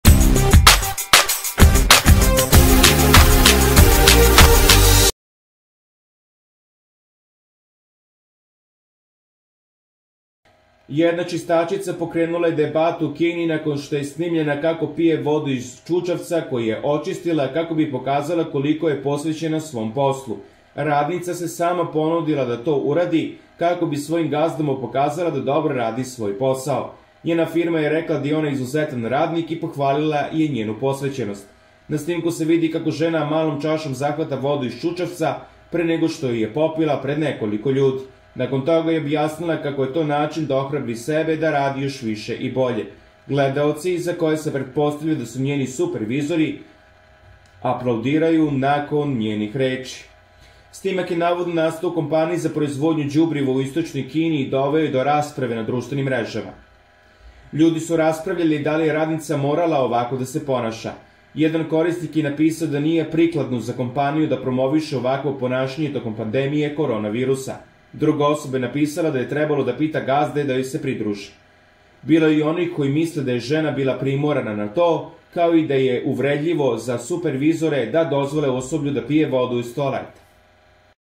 Desio se UZAS! Čistačica MORALA da pije vodu iz ČUČAVCA da pokaže kako je dobro obavila posao!
Jedna čistačica pokrenula je debatu u Kini nakon što je snimljena kako pije vodu iz čučavca koji je očistila, kako bi pokazala koliko je posvećena svom poslu.